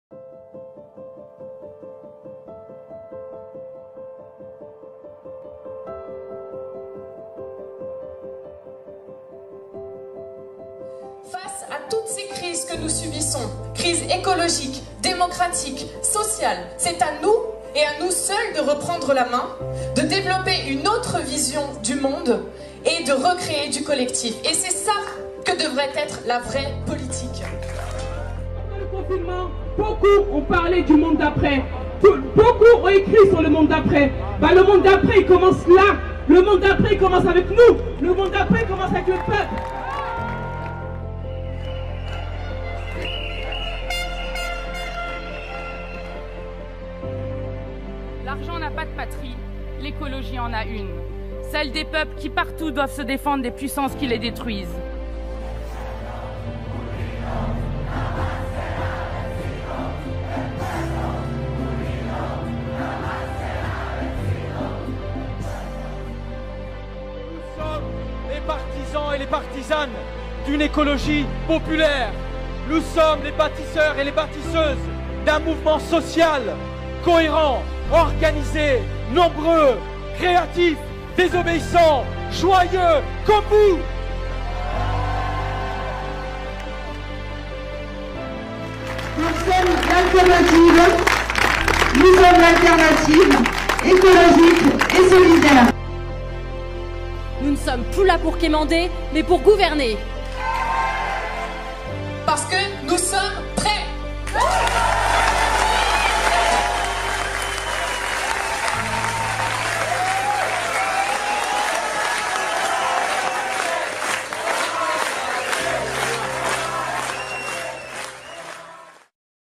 LE TEMPS DU POUVOIR HABITANT - Rencontres nationales du pouvoir habitant
Les 12, 13 et 14 novembre 2021, à Nantes, auront lieu les Rencontres nationales du pouvoir habitant.

Face aux crises de notre époque, nous nous sentons dépossedés de notre pouvoir : il est temps de nous le réapproprier ! Construction de communs, mouvements municipalistes, luttes habitantes, ... partout, nous reprenons en mains nos vies et nos lieux de vie.

Trois jours d'échanges entre celles et ceux qui agissent et pensent la réappropriation du pouvoir par les habitantes et les habitants.